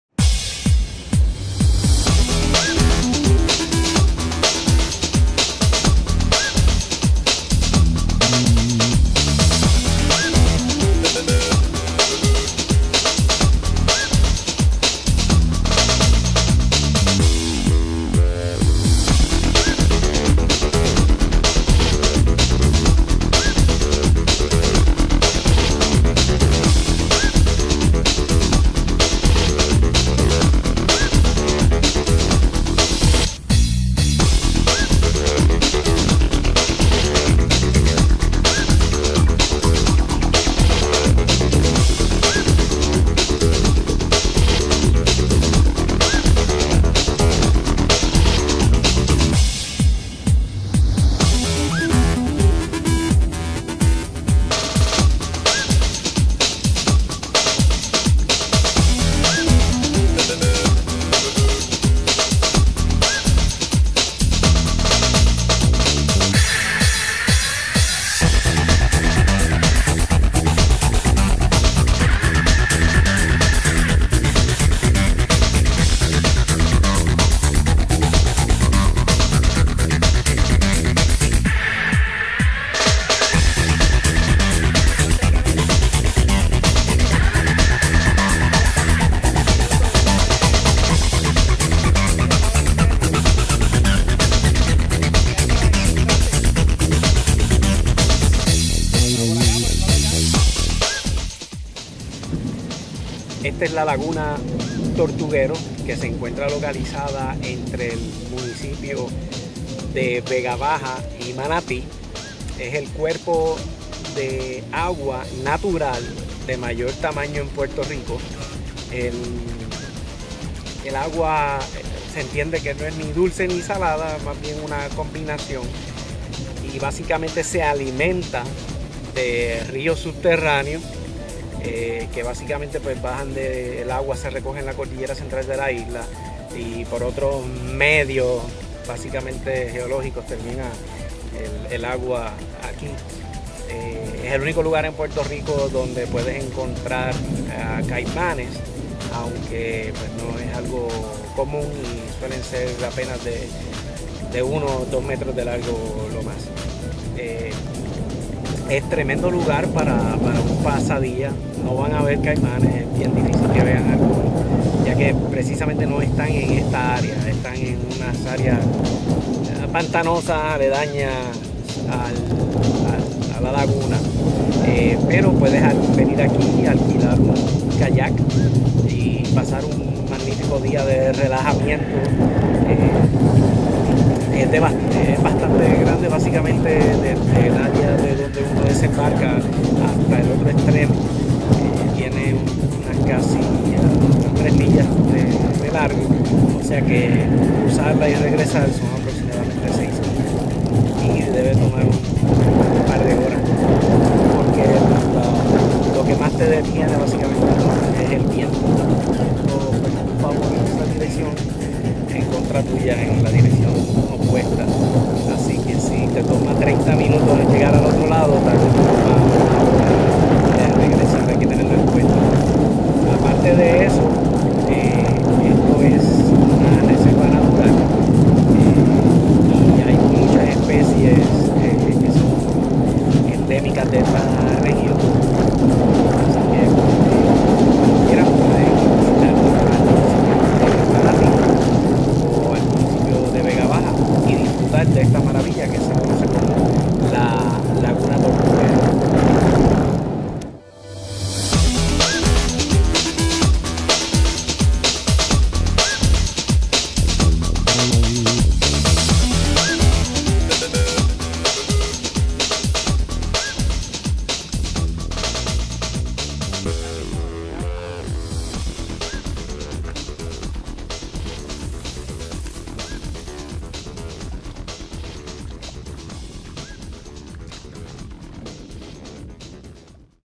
Laguna Tortuguero
Located between the towns of Vega Baja and Manatí, Turtuguero is Puerto Rico's largest natural body of water. It is also a wildlife reserve and it spans almost 4.5 kilometers. Its shallow waters are mostly 1.5 meters deep. A lot of endemic species make Tortuguero their home and small caimans also live in the area although encounters are extremely rare as they avoid people. Fishing and kayaking are the most common activities in the lagoon.